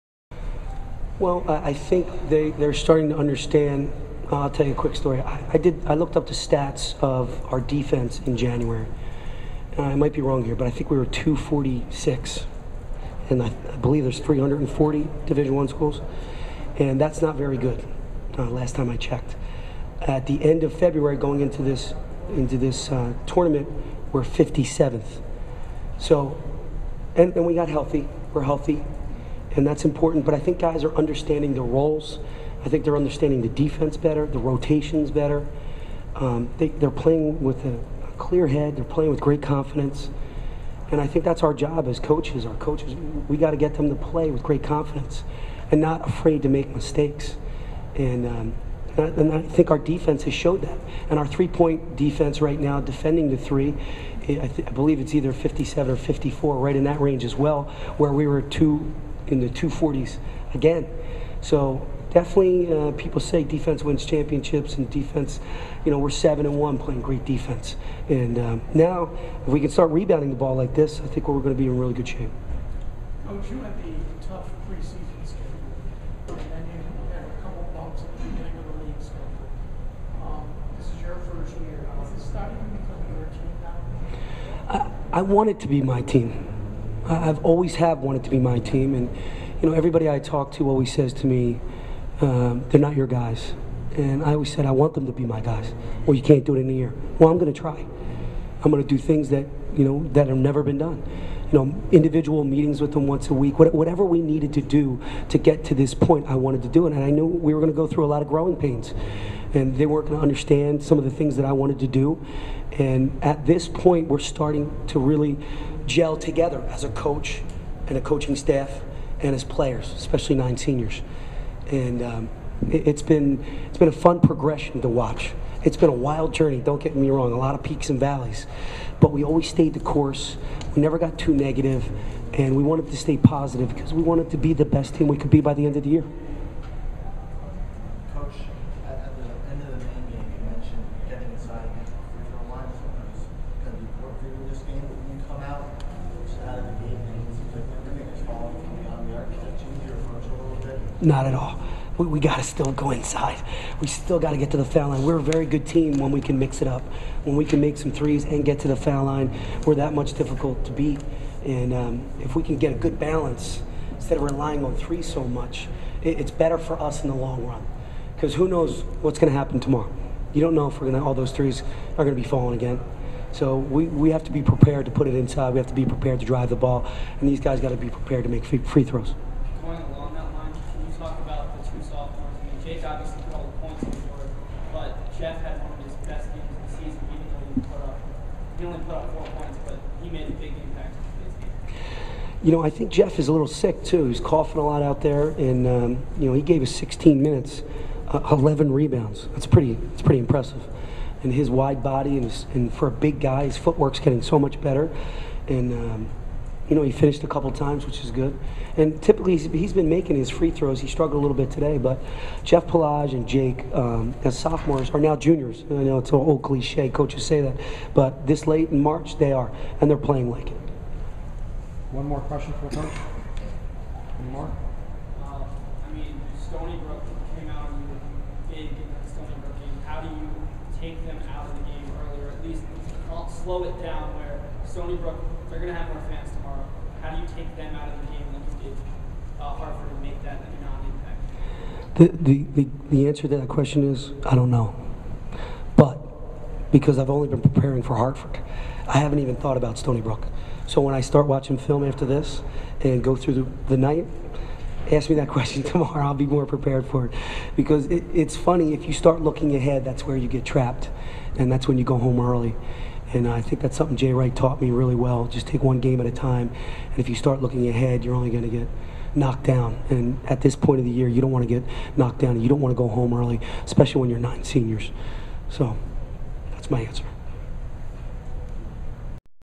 BU Men's Basketball/Hartford Postgame Comments III
Boston University men's basketball head coach Pat Chambers talks to the media following the Terriers' 87-46 victory over Hartford.